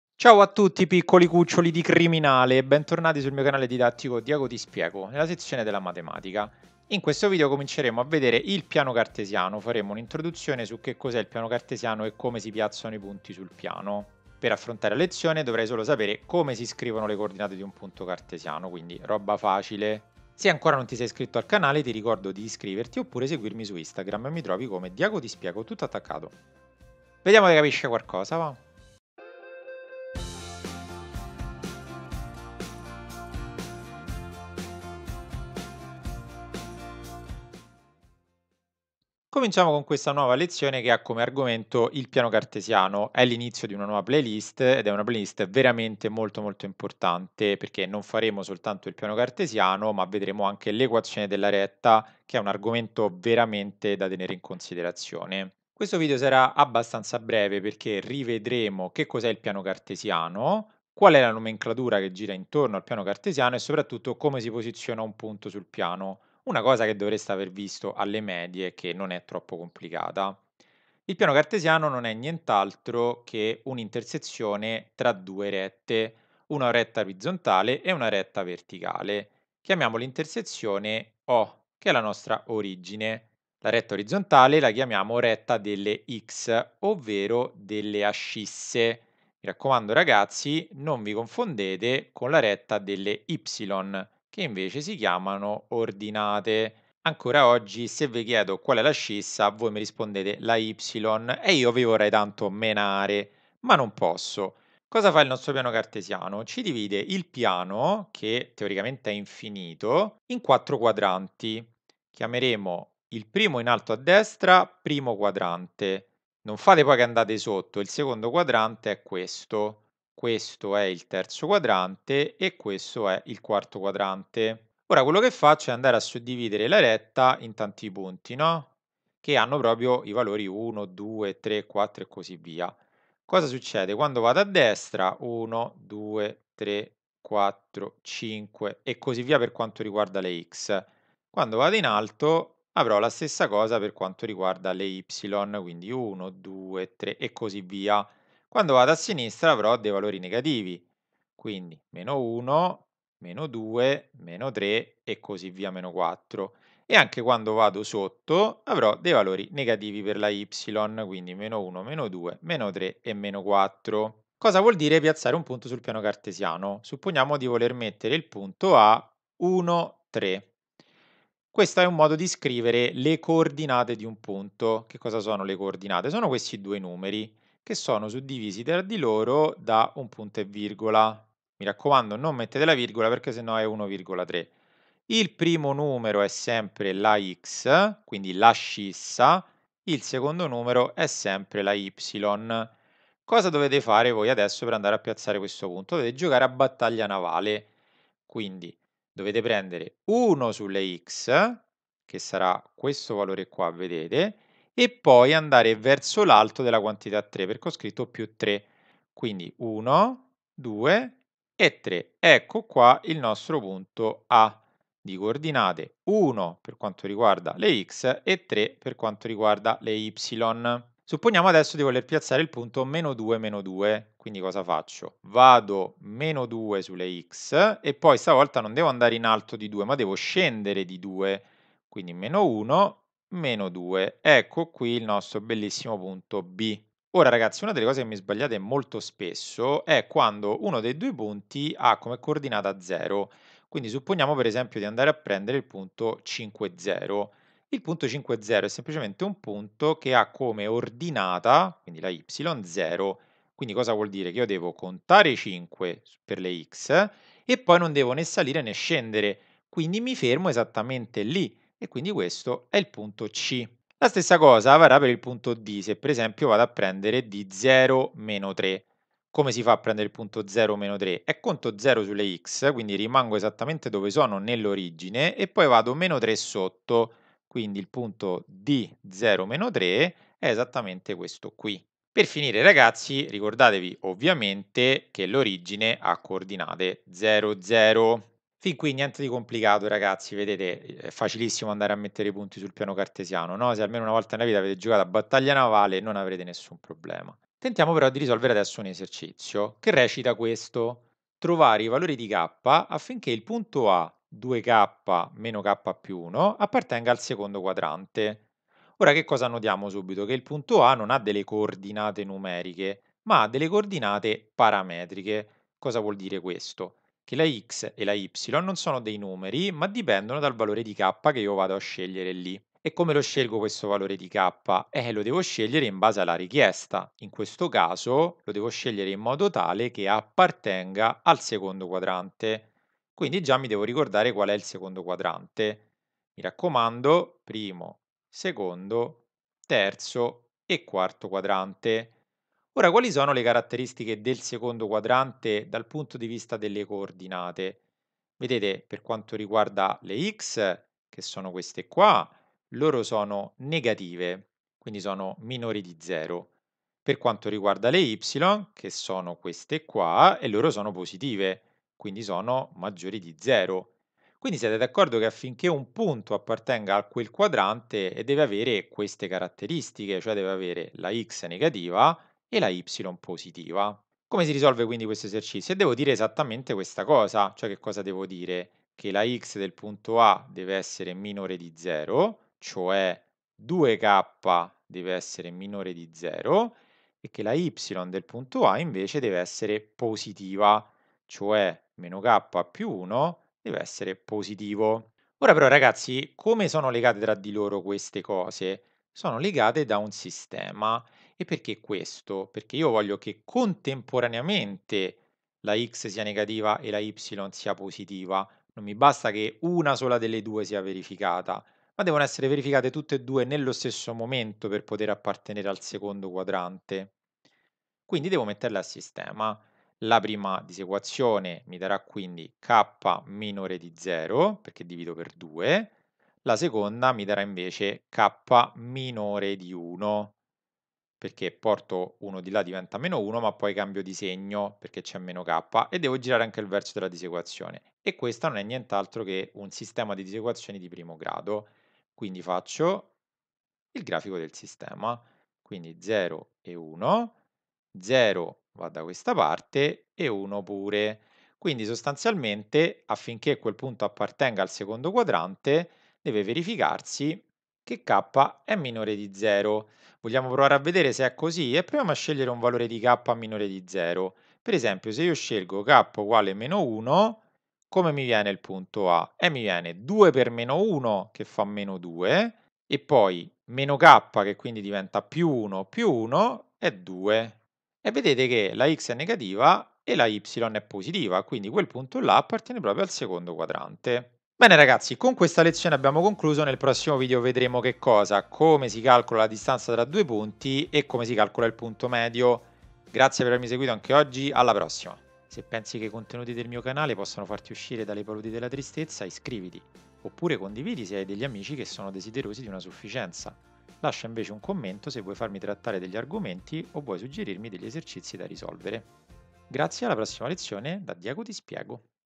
IL PIANO CARTESIANO E LA RETTA: introduzione al piano cartesiano
In questa video lezione ripasseremo il piano cartesiano già visto alle medie.
Dopo aver rivisto la nomenclatura ed in particolare quali sono le ascisse e le ordinate e quali sono i quadranti, vedremo come posizionare vari punti sul piano.
Infine vedremo un esercizio di ragionamento.